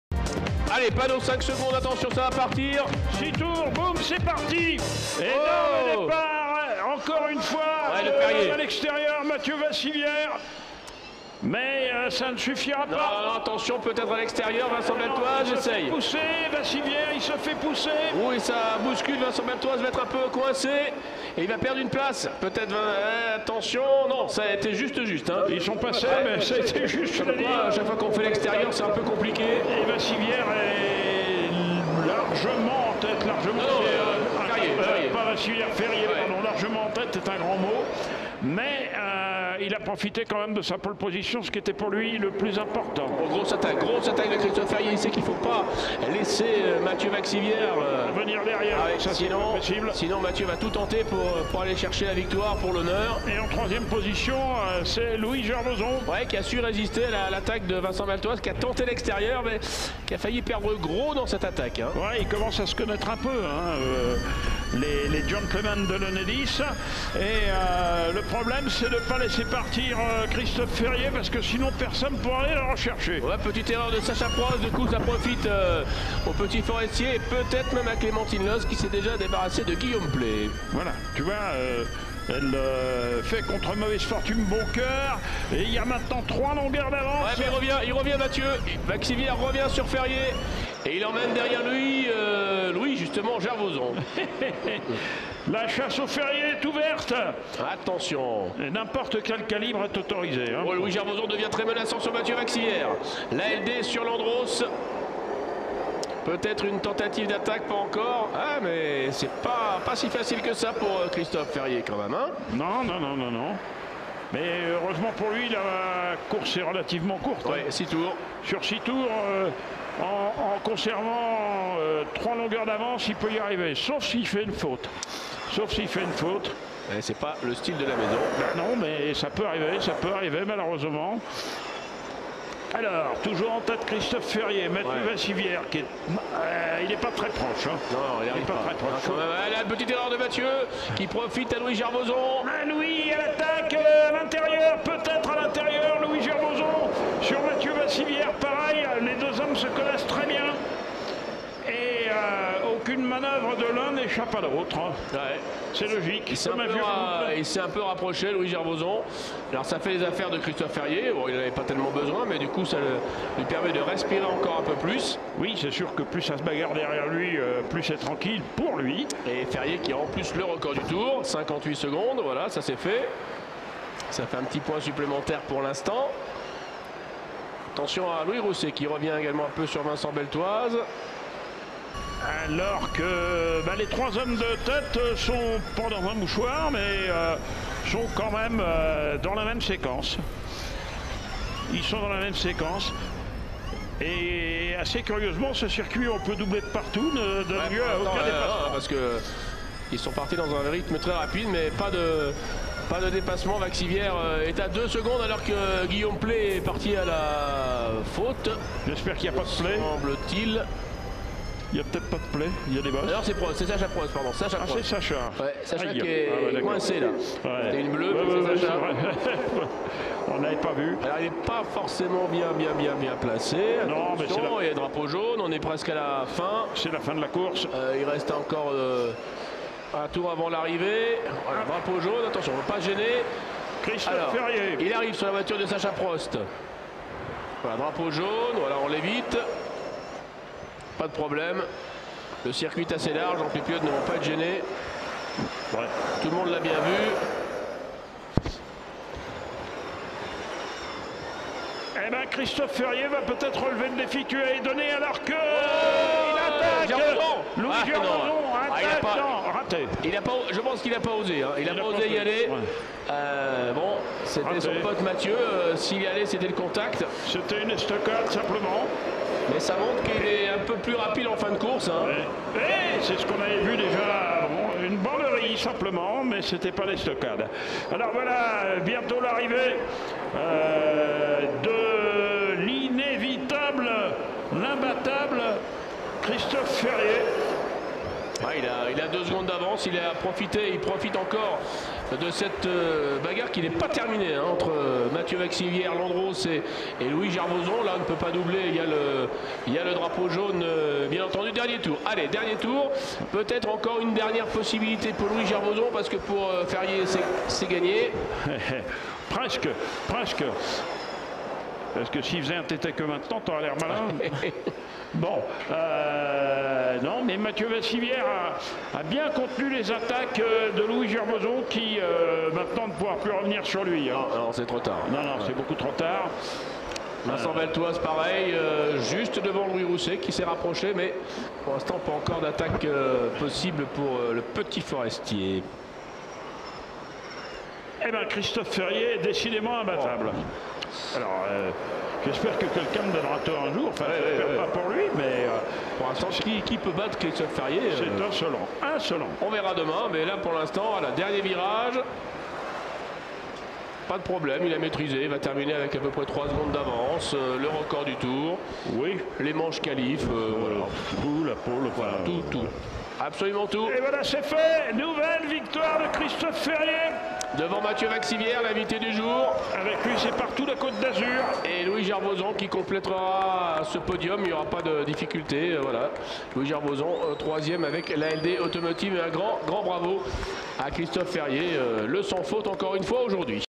FINALE ENEDIS Trophée Andros Electrique Course 1 - Serre Chevalier 2019
Christophe Ferrier, vainqueur Haut-Alpin !

Les pilotes de l’Enedis Trophée Andros Electrique ont de nouveau offert le spectacle à un public venu nombreux.

Christophe Ferrier (Métropole Nice Côte D'azur) n’a une fois de plus rien laissé à ses adversaires en remportant les manches qualifs et en s’octroyant la finale. Matthieu Vaxivière (Andros) très à l’aise sur le circuit de Serre Chevalier s’offre la deuxième place devant Louis Gervoson (ALD Automotive).